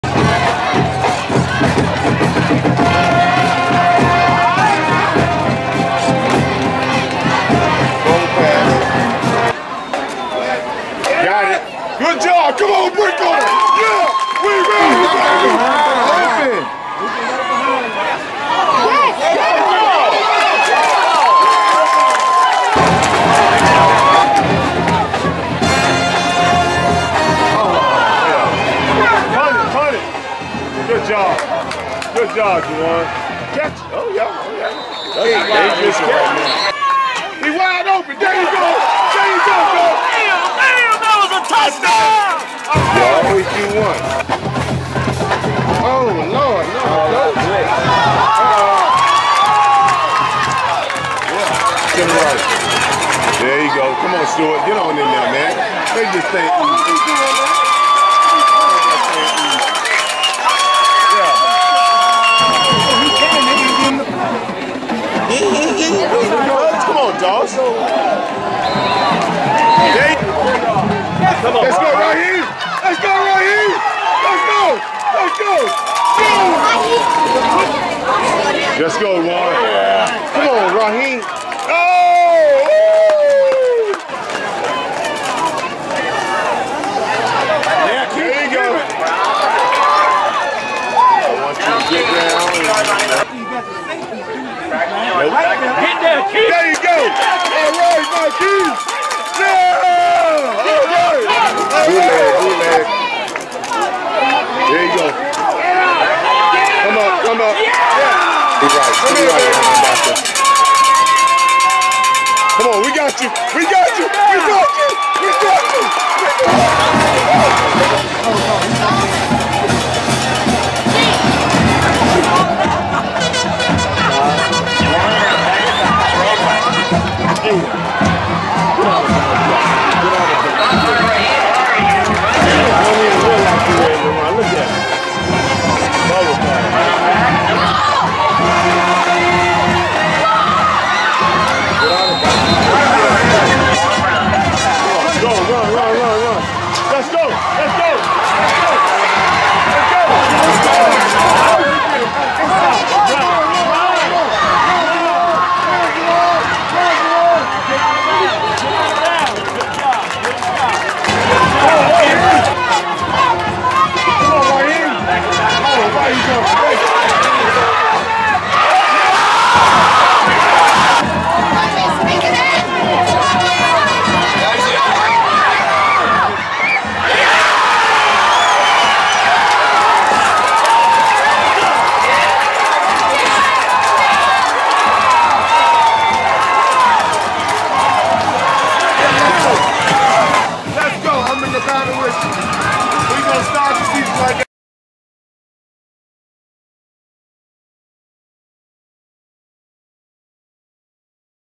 2012 Largo Packers vs Bradenton Southeast Highlights [No Music]
2012 Largo Packers avenge last season's loss to The Bradenton Southeast Seminoles